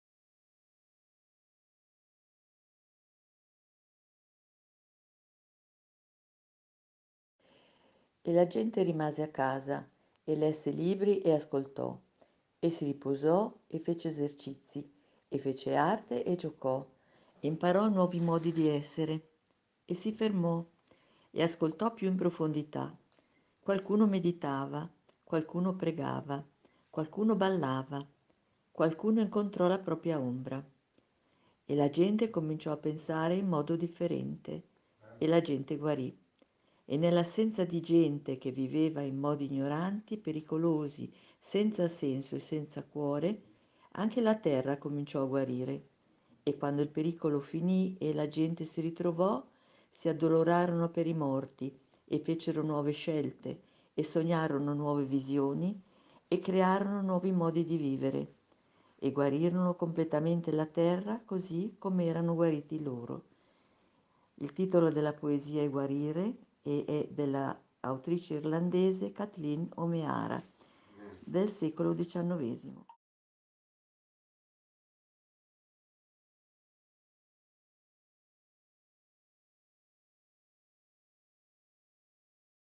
Guarire - Kathleen O'Meara -
Letta da Isabella Somaglia.
Guarire di Kathleen O'Meara. Questa è una poesia, scritta da Kitty O' Meara il 16 marzo 2020, pubblicata dalla stessa autrice sul suo blog, The Daily Round.
Buon ascolto!